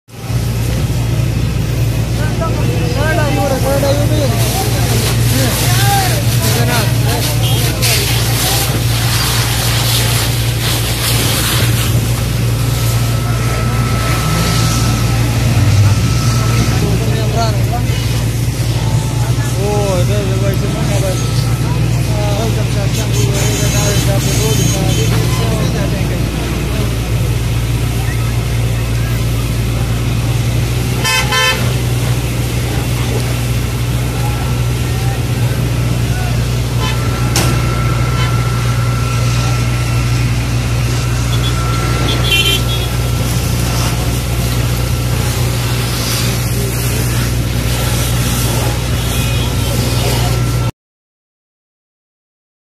ایوب پارک کار کو آگ لگ گئی، جانی نقصان نہیں ہوا
راولپنڈی ایوب پارک کار کو آگ لگ گئی، گاڑی جل کر خاکستر ہو گئی۔ ریسکیو 1122 کے عملہ نے آگ پر قابو پا لیا۔ تاہم کوئی جانی نقصان نہیں ہوا۔